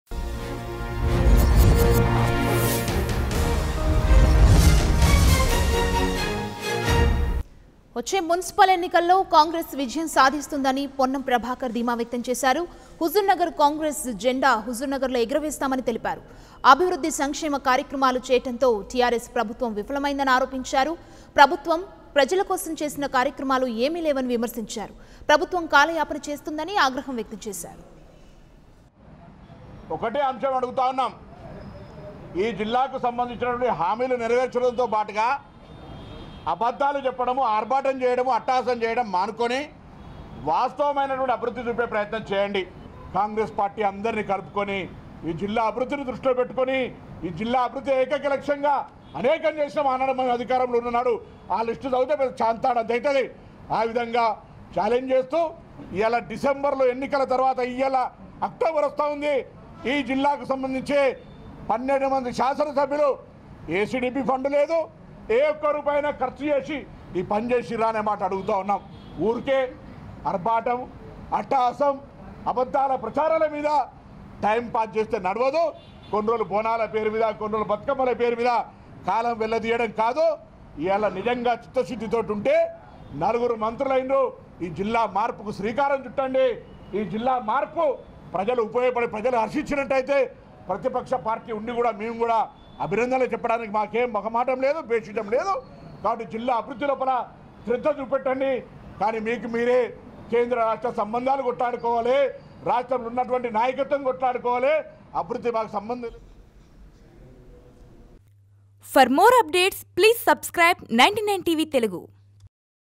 హుజుర్ నగర లో జెండా ఎగరవేస్తాం | Ponnam Prabhakar About Huzurnagar By Poll | Congress vs TRS | 99TV
హుజుర్ నగర లో జెండా ఎగరవేస్తాం | Ponnam Prabhakar About Huzurnagar By Poll | Congress vs TRS.The news of 99TV Telugu is daring, straightforward and biased only towards people.